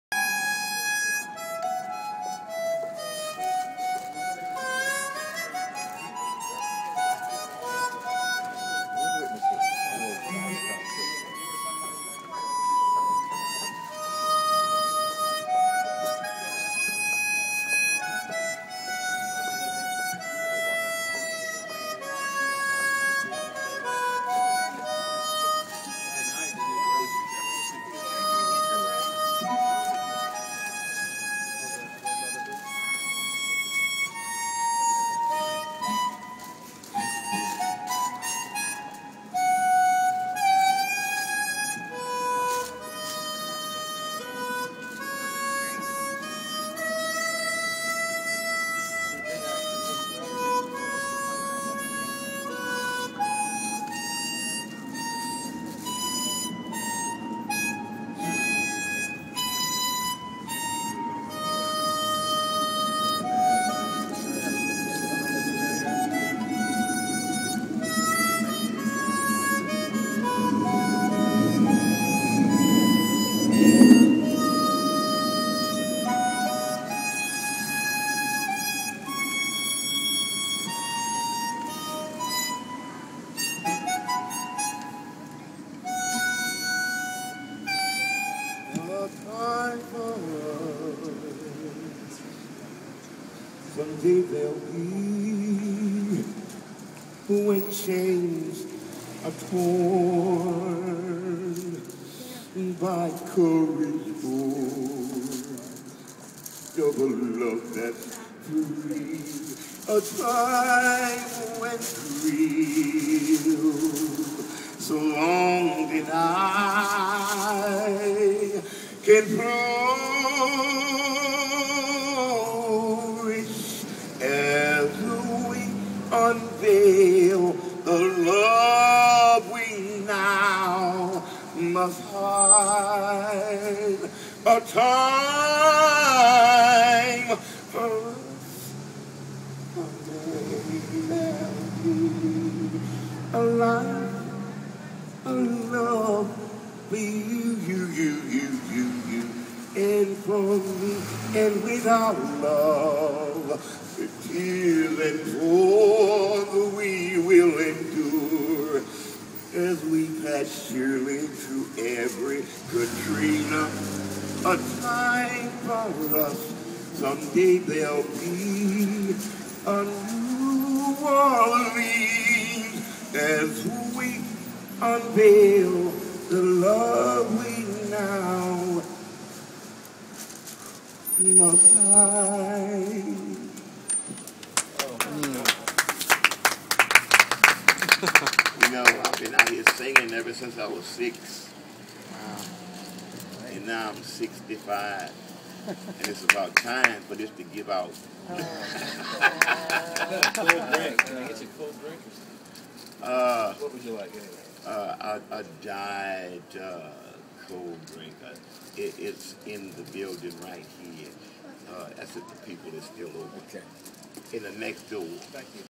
Grandpa Elliott on the street pt 2
Grandpa Elliott on a random street in new Orleans. We just happened to walk by and see him. This is the end of his medley.